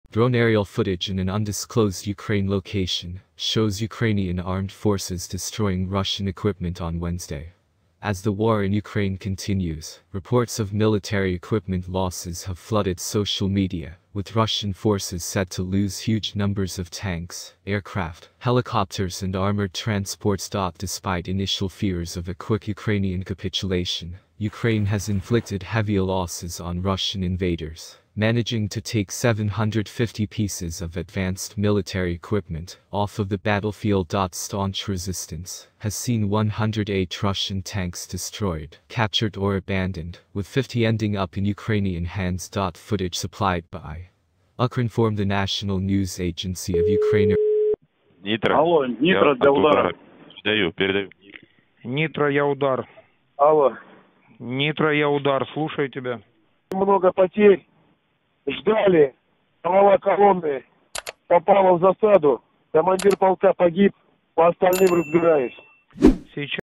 Russian tank destroyed by Ukrainian army in Kyiv
Drone aerial footage in an undisclosed Ukraine location shows Ukrainian armed forces destroying Russian equipment on Wednesday.

As the war in Ukraine continues, reports of military equipment losses have flooded social media with Russian forces said to lose huge numbers of tanks, aircraft, helicopters and armoured transports.

Despite initial fears of a quick Ukrainian capitulation, Ukraine has inflicted heavy losses on Russian invaders, managing to take 750 pieces of advanced military equipment off of the battlefield.

Staunch resistance has seen 108 Russian tanks destroyed, captured or abandoned with 50 ending up in Ukrainian hands.

Footage Supplied By: Ukrinform - The National News Agency of Ukraine or Ukrinform is a state information and news agency of Ukraine.